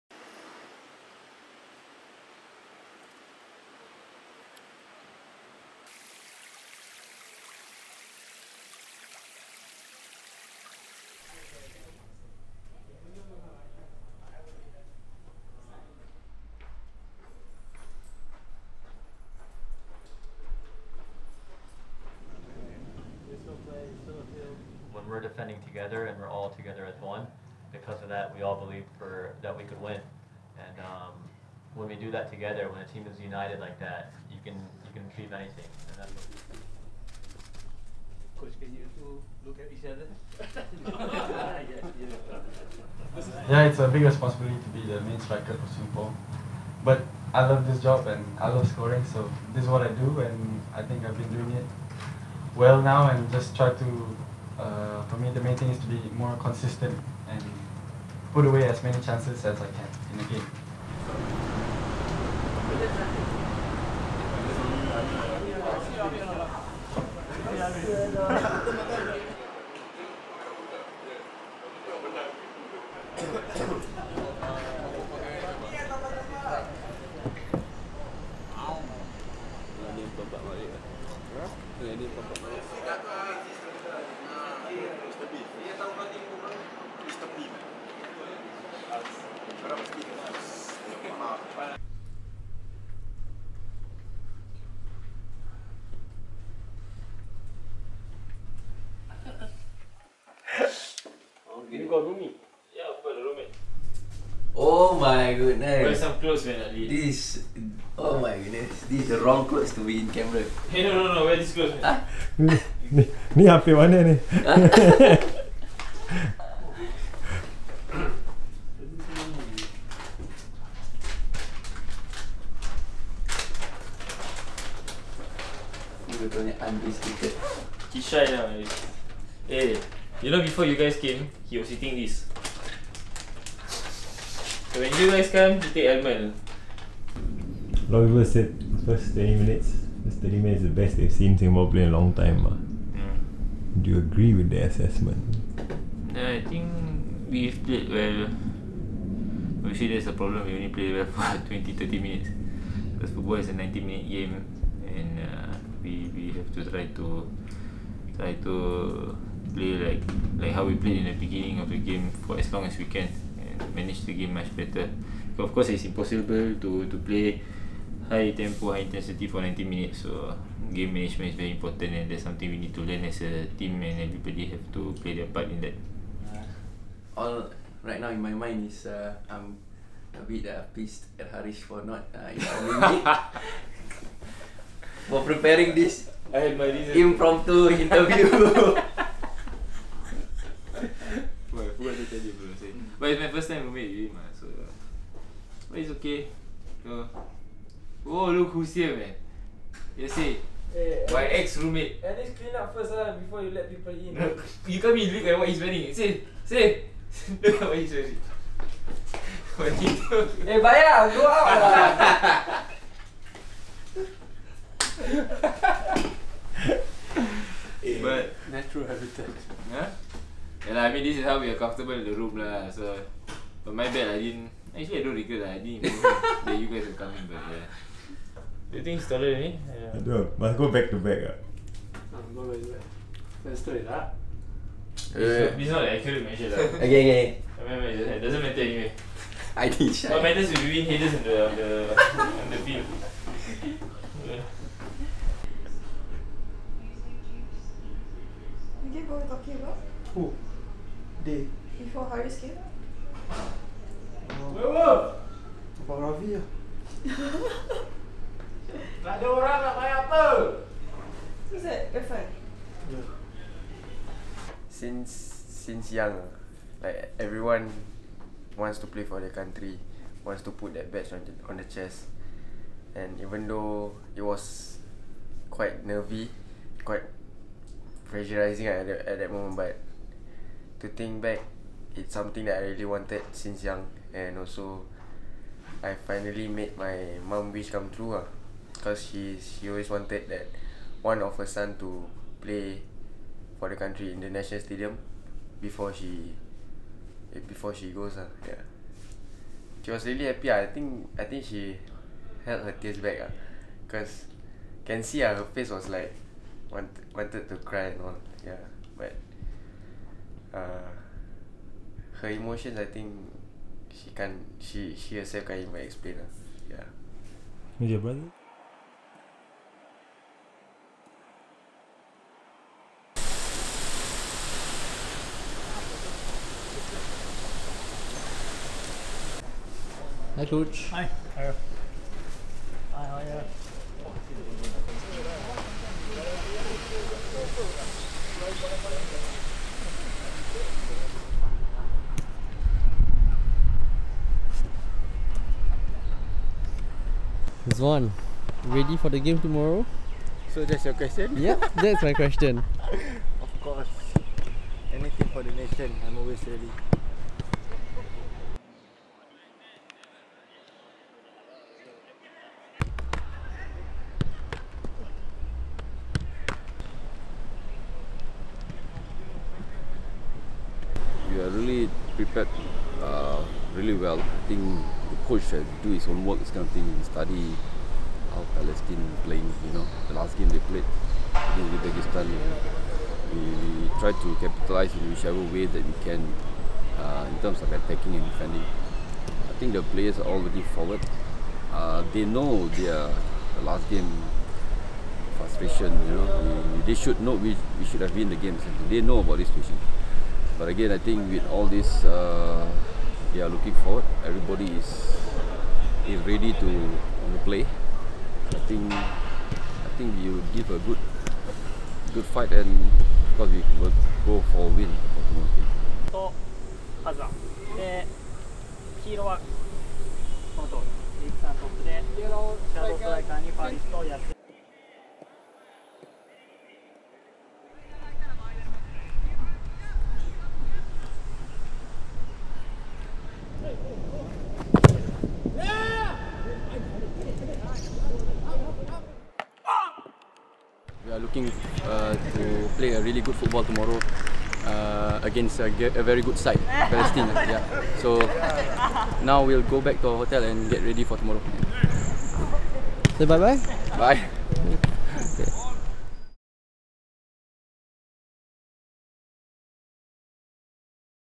Lions Unrestricted: Preparing for Palestine | 2022 FIFA World Cup Qualifiers
What do our Lions get up to in their room? Hariss and Hami let us in for a peak as they gear up to play Palestine!